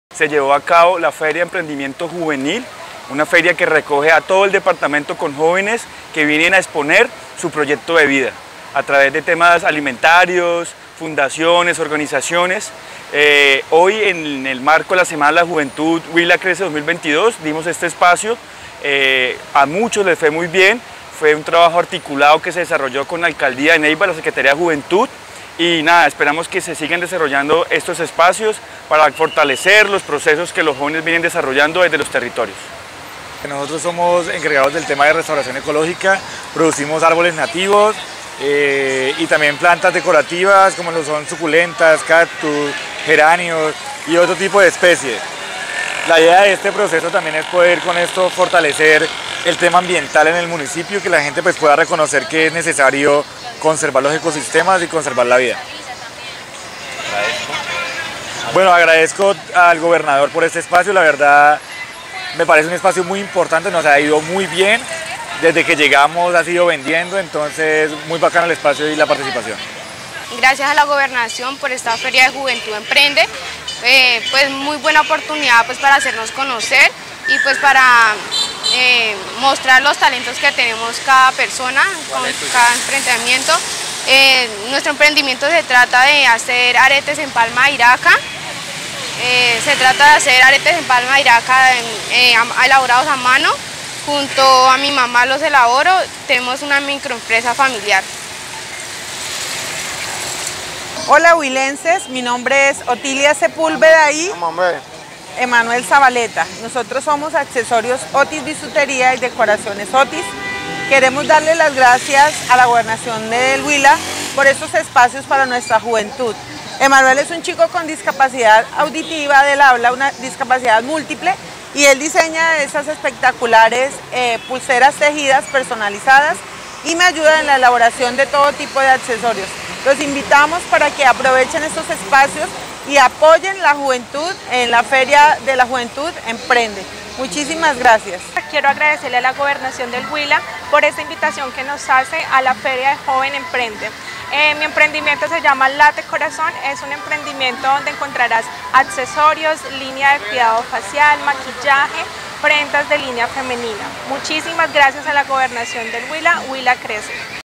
Feria Juventud Emprende
En el marco de la Semana Departamental de la Juventud, Gobierno Huila Crece abrió espacios para la promoción del talento empresarial y la innovación de jóvenes de distintos Municipios del Departamento